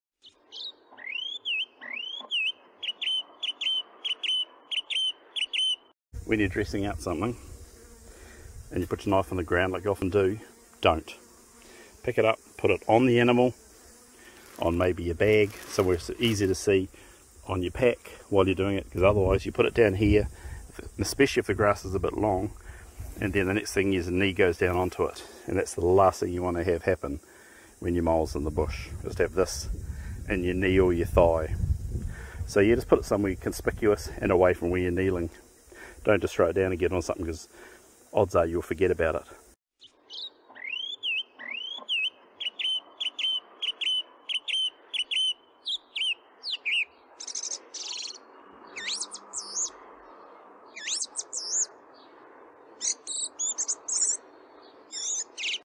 Hunting tip #2 How to NOT cut yourself when hunting in the bush.
How to NOT cut yourself when hunting in the bush.